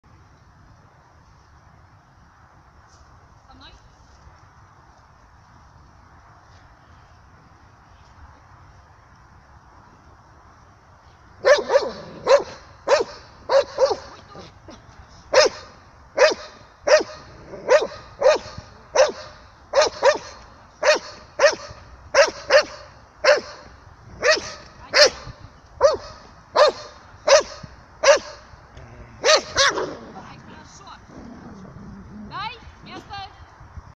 Малинуа. Охрана хозяина.
Первая тренировка по охране владельца.